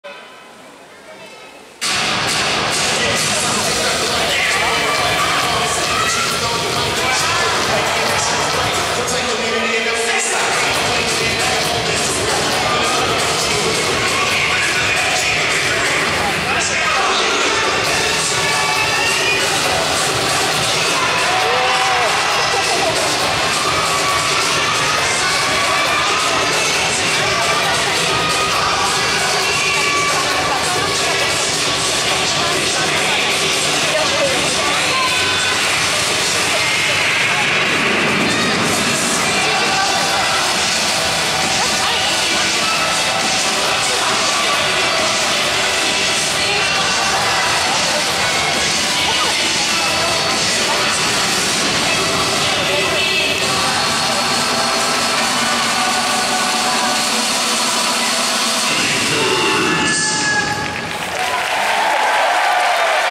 クラブチームVIPERS ダブルス ジャパンカップ 2019.08.25
JAPAN CUP 2019 チアリーディング日本選手権大会 ダブルス演技競技 決勝